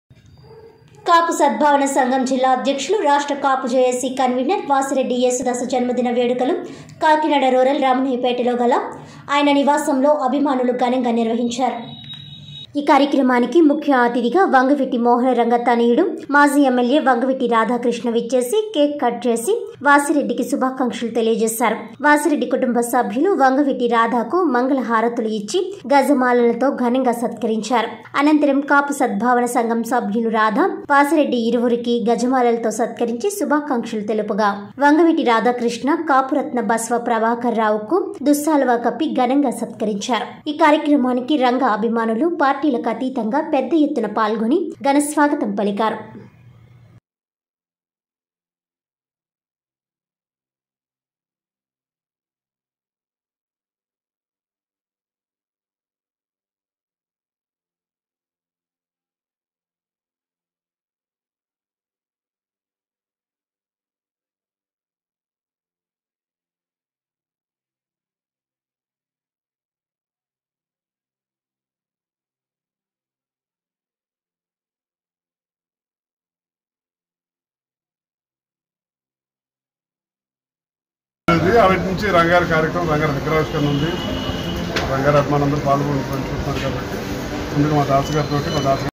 కాకినాడ రూరల్ -|15-07-2022|TV 7 NEWS
కాపు సద్భావన సంఘం జిల్లా అధ్యక్షులు రాష్ట్ర కాపు జేఏసీ కన్వీనర్ వాసిరెడ్డి ఏసుదాసు జన్మదిన వేడుకలు కాకినాడ రూరల్ రమణయ్యపేటలో గల ఆయన నివాసంలో అభిమానులు ఘనంగా నిర్వహించారు.